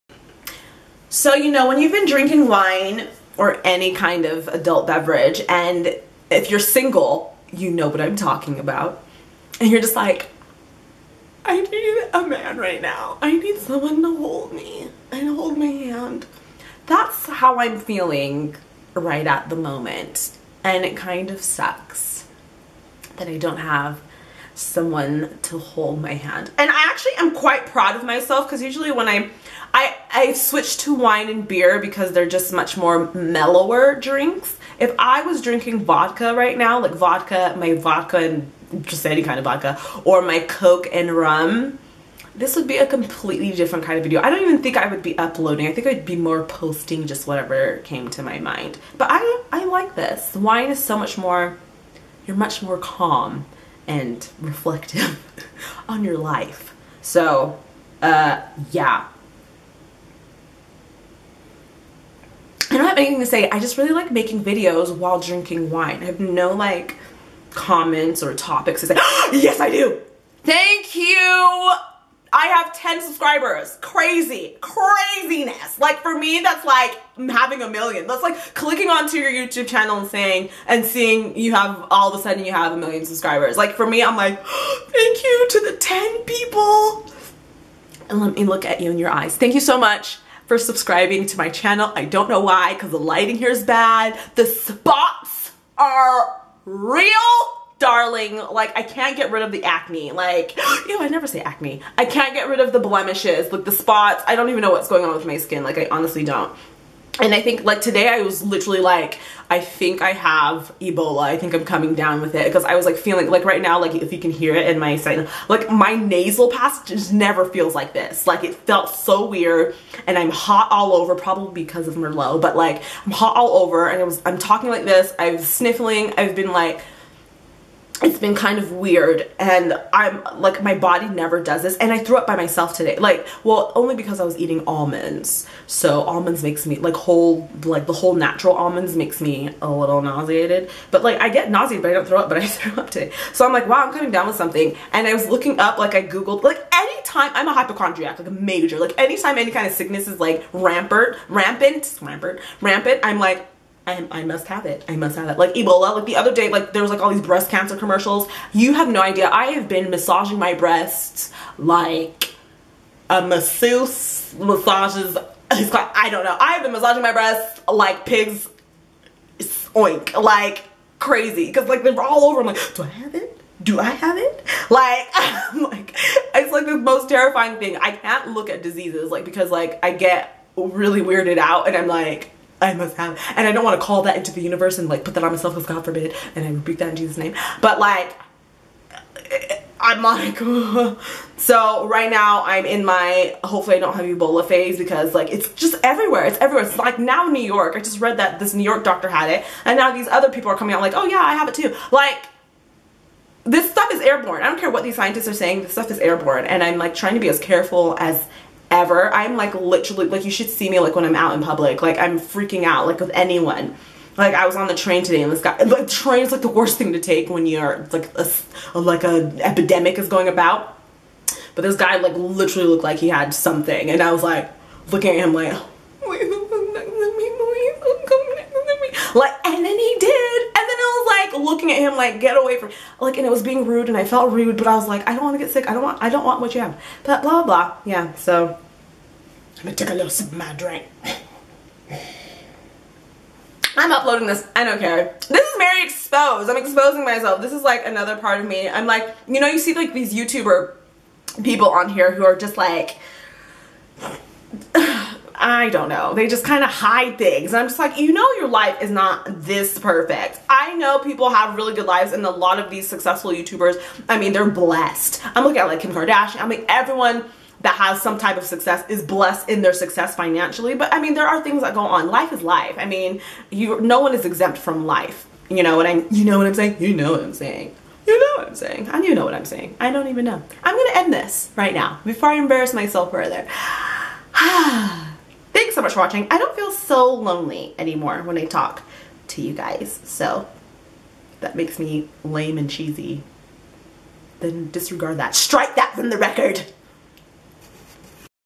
10 Subscribers!!! now 8 ......:/...THANK YOU
THANK YOU SOOOOOOOOOOO much to the 8 out of 10 subbies that stayed... I am so grateful to you.